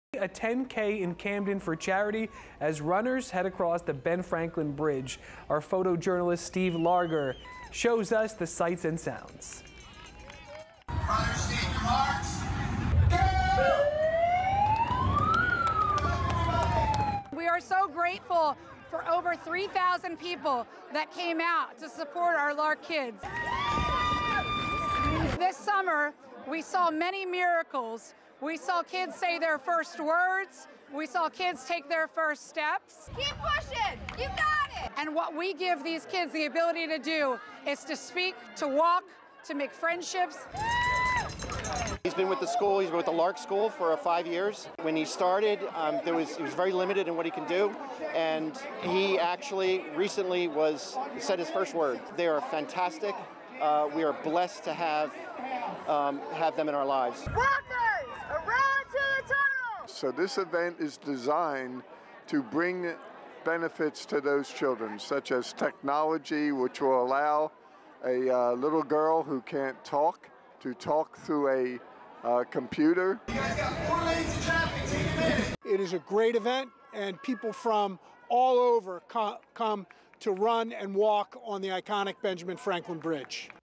Thousands of runners cross the Ben Franklin Bridge to raise money for kids with special needs
The event raised money for the Larc School, a private nonprofit special education school that serves students with disabilities.